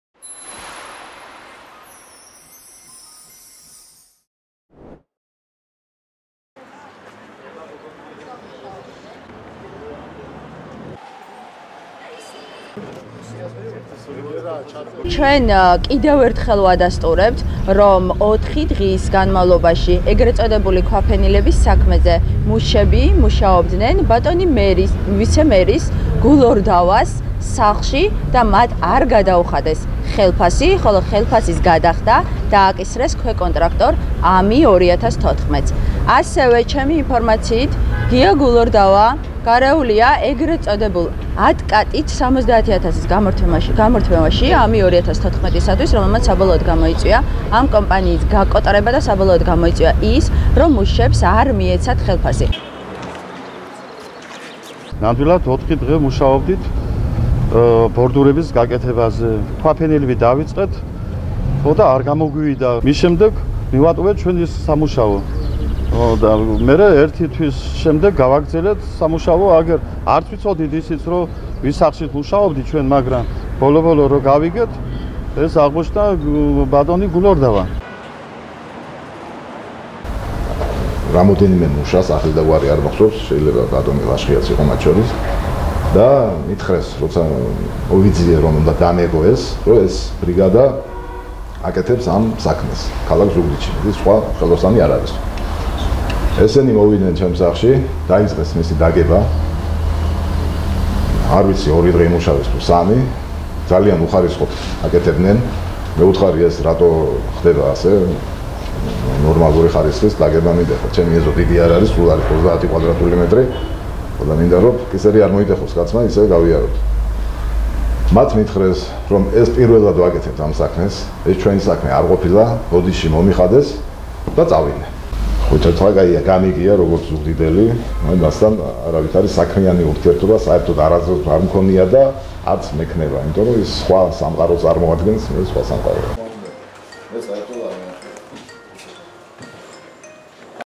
ანა წითლიძემ ვიცე მერის გია გულორდავას გუშინდელ განცხადებას გამოეხმაურა
ნაცონალური მოძრაობის" წევრი ანი წითლიძე ვიცე-მერის გია გულორდავას გუშინდელ განცხადებას გამოეხმაურა.
ზუგდიდის პროკურატურასთან ანი წითლიძემ ბრიფინგი მუშებთან ერთად გამართა და გია გულორდავას მიმართ გამოთქმული ბრალდებები დაადასტურა, რომელთა თანახმად, ის ზუგდიდის ტროტუარის მოპირკეთების სამუაშოებზე დაკავებულ მუშებს, ოთხი დღის განმავლობაში საკუთარ სახლში ამუშავებდა.

გარდა ამისა, დეპუტატის განცხადებით, გულორდავამ მუშებისთვის ხელფასების გადახდა „ამი2014“-ს დააკისრა.

დეპუტატის თქმით გია გულორდავა ასევე, გარეულია ფინანსურ გარიგებაში, რამაც „ამი2014“-ს გაკოტრება გამოიწვია, შესაბამისად კომპანიამ მუშებს თანხა ვერ გადაუხადა.

გია გულორდავა ადასტურებს იმ ფაქტს, რომ რამდენიმე დღის განმავლობაში მასთან დასაქმებულები იყვნენ მუშები, რომლებმაც სამუშაოს ვერ გაართვეს თავი და მალევე შეწყვიტეს.

ვიცე-მერის თქმით განხორციელებულ სამუშაოზე, მუშებისთვის განკუთვნილი ხელფასი მათ ხელმძღვანელს გადაუხადა. რაც შეეხება ფინანსურ გარიგებაში მის მონაწილეობას, არ ადასტურებს და აბსურდულს უწოდებს.